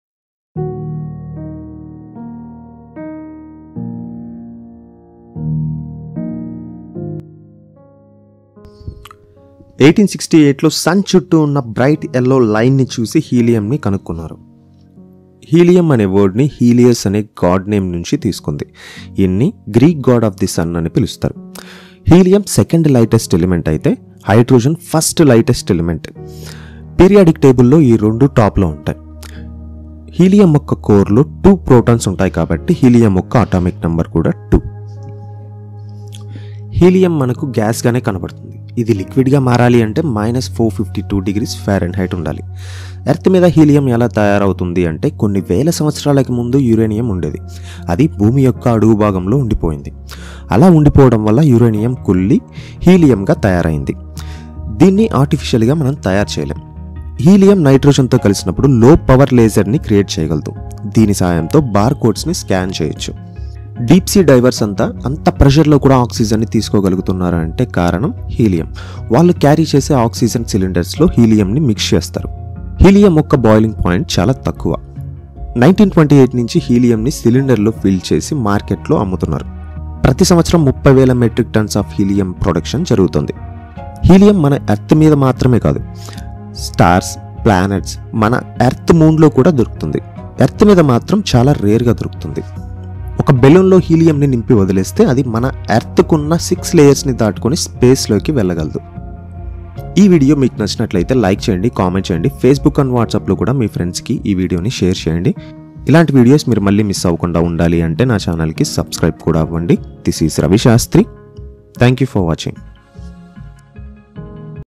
HELIUM FACTS IN TELUGU BY RAVI SHASTRY
HELIUM FACTS IN TELUGU
HELIUM WAS DISCOVERED IN 1868.HELIUM NAME WAS TAKEN FROM GEEK GOD CALLED ''HELIOS'' HE CALLED AS GEEK GOD OF THE SUN. HELIUM WAS 2nd LIGHTEST ELEMENT IN THE UNIVERSE 1st LIGHTEST ELEMENT WAS HYDROGEN.